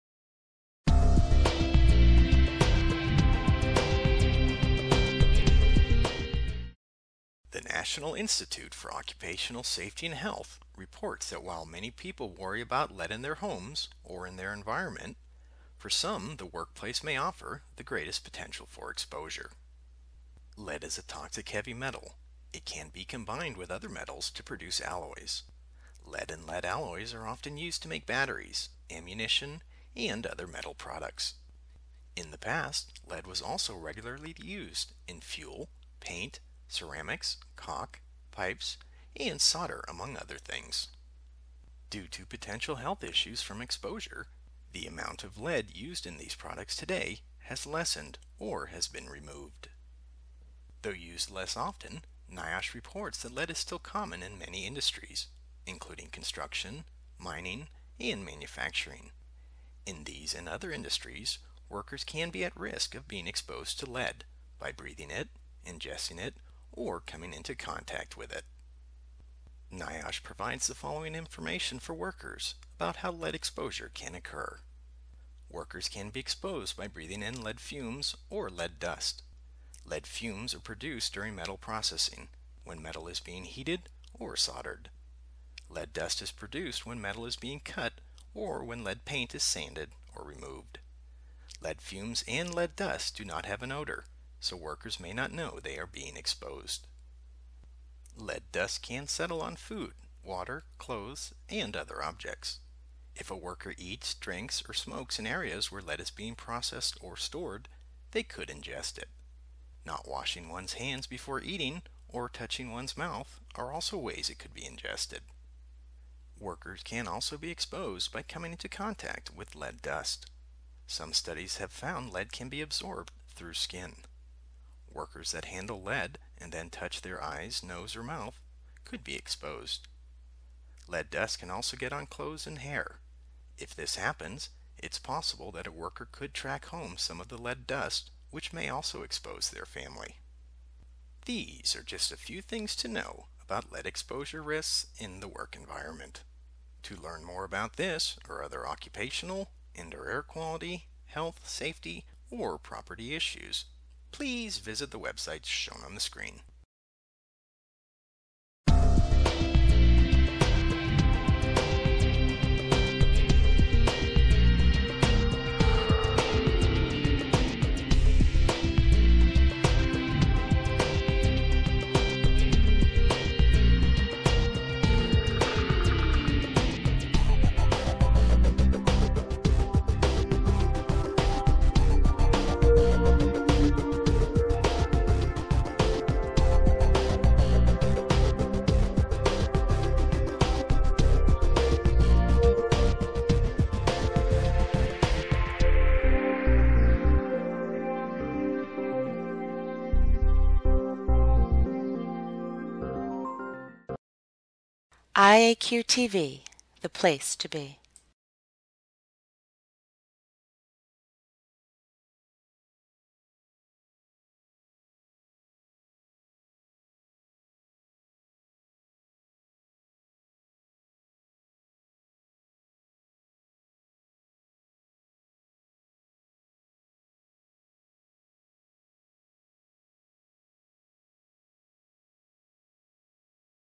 Lead Exposure Risks in the Work Environment
The National Institute for Occupational Safety and Health (NIOSH) reports that while many people worry about lead in their homes or in the environment, for some the workplace may offer the greatest potential for exposure.

Lead is a toxic heavy metal. It can be combined with other metals to produce alloys. Lead and lead alloys are often used to make batteries, ammunition and other metal products. In the past, lead was also used regularly in fuel, paint, ceramics, caulk, pipes and solder among other things.

Due to potential health issues from exposure, the amount of lead used in these products today has lessened or has been removed. Though used less often, NIOSH reports that lead is still common in many industries, including construction, mining and manufacturing. In these and other industries, workers can be at risk of being exposed to lead, by breathing it, ingesting it or coming in contact with it.

NIOSH provides the following information for workers about how lead exposure can occur:

• Workers can be exposed by breathing-in lead fumes or lead dust. Lead fumes are produced during metal processing, when metal is being heated or soldered. Lead dust is produced when metal is being cut or when lead paint is sanded or removed. Lead fumes and lead dust do not have an odor, so workers may not know they are being exposed.

• Lead dust can settle on food, water, clothes and other objects. If a worker eats, drinks or smokes in areas where lead is being processed or stored, they could ingest it. Not washing one’s hands before eating or touching one’s mouth are also ways it could be ingested.

• Workers can also be exposed by coming into contact with lead dust. Some studies have found lead can be absorbed through skin. Workers that handle lead and then touch their eyes, nose or mouth, could be exposed. Lead dust can also get on clothes and hair. If this happens, it’s possible that a worker could track home some of the lead dust, which may also expose their family.

These are just a few things to know about lead exposure risks in the work environment. To learn more about this or other occupational, indoor air quality, health, safety or property issues, please visit the websites shown on the screen.